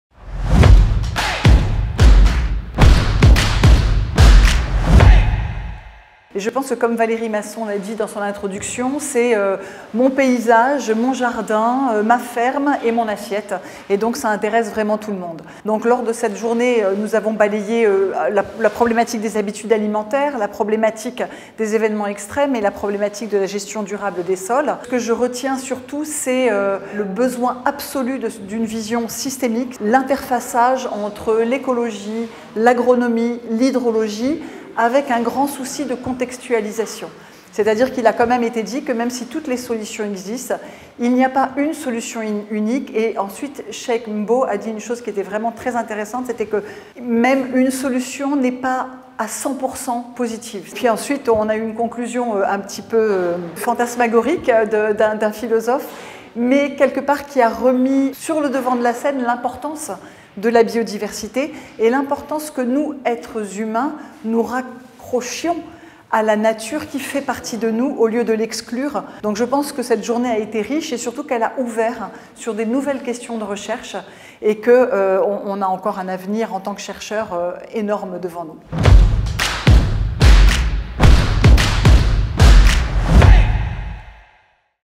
Interview de Nathalie de NOBLET (IPSL-LSCE)
"Que retenir de cette Journée Scientifique ?"
Interview de Nathalie de NOBLET, présidente du Comité Scientifique de la Journée 2019 "Changement climatique, gestion durable des terres et sécurité alimentaire”, organisée par Météo et Climat le 28 novembre 2019 à l’Hôtel de l’Industrie, Paris 6e